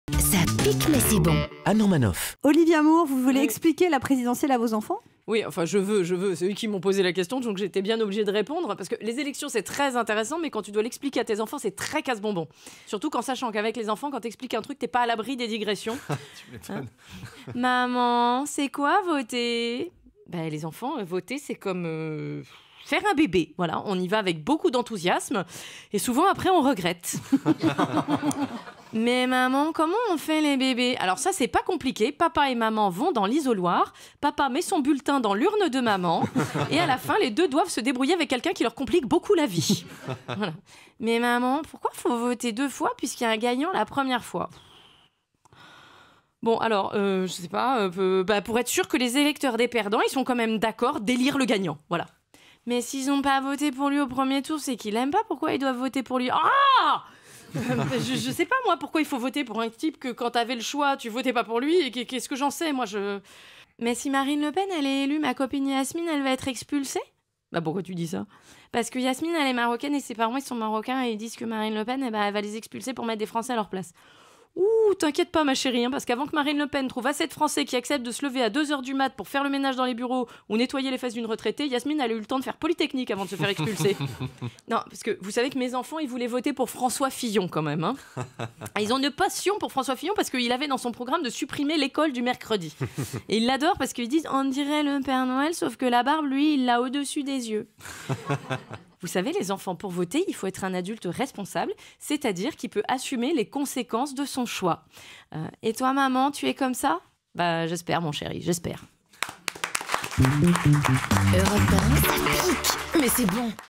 Olivia Moore : "La présidentielle expliquée à mes enfants"
Les enfants d'Olivia Moore lui ont demandé des explications sur la présidentielle et le moins que l'on puisse dire c'est que ce n'est... pas évident.